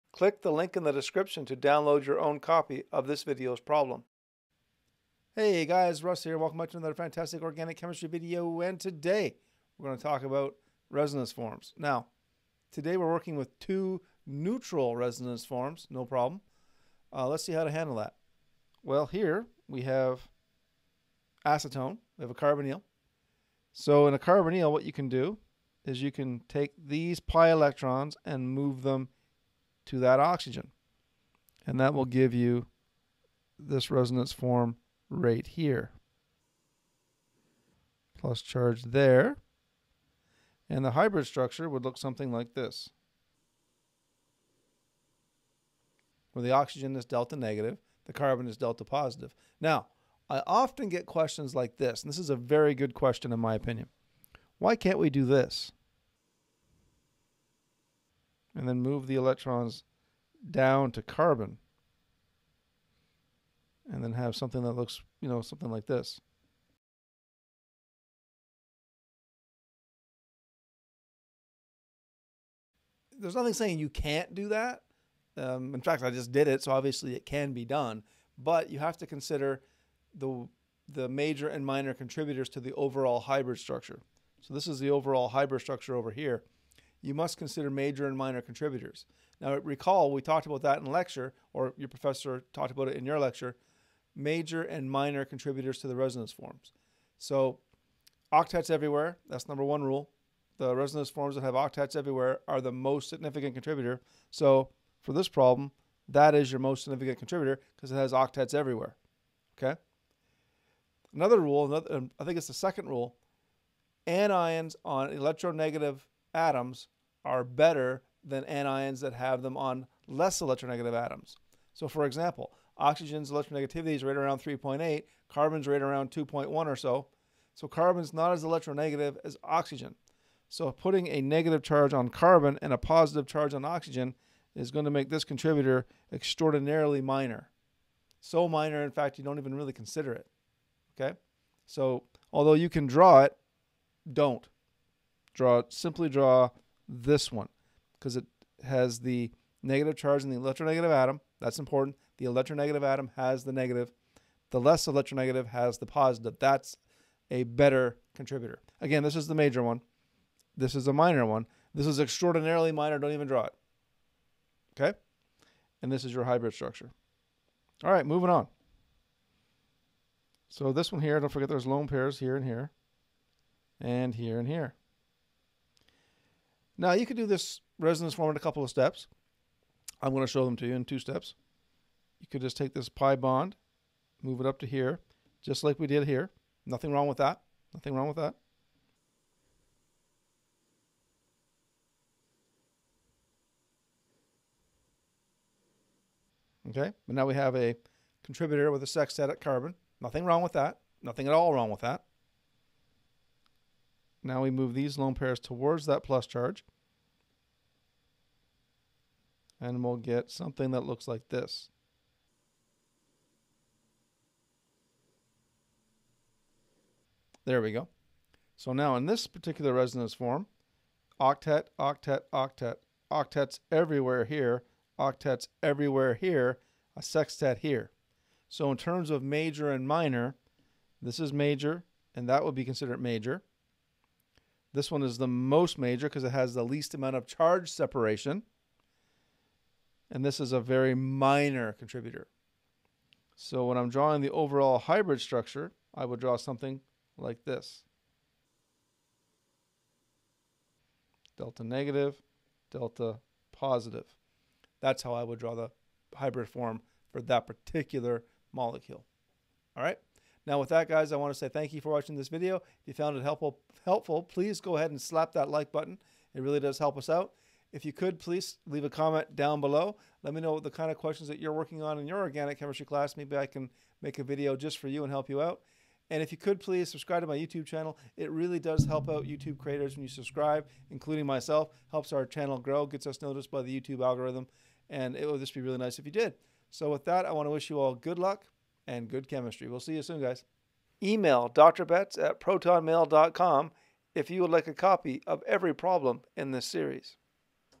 Resonance of Ketones and Esters (Carbonyls) - Organic Chemistry Resonance Practice
Resonance forms of carbonyl containing molecules are extremely important concepts in organic chemistry. This video will go over the resonance of a ketone and an ester. If you are able to draw the correct resonance forms of an carbonly containing compounds you can predict the reactivity of a molecule by knowing where an partial negative (delta negative) and partial positive (delta positive) are located on a molecule.

Resonance involves arrow pushing.  Arrow pushing involves the movement of electrons. For resonance arrow pushing involves the pi electrons only.  It is very important that you become skilled at pushing arrows for resonance as it will be a valuable skill when you move into mechanisms.

I recommend that you download the problem from the link below and attempt the problem yourself and use this video to correct your work.

Good Luck and Good Chemistry!

00:00 Introduction
00:20 Resonance of Ketone
02:48 Resonance of an Ester
04:20 Conclusion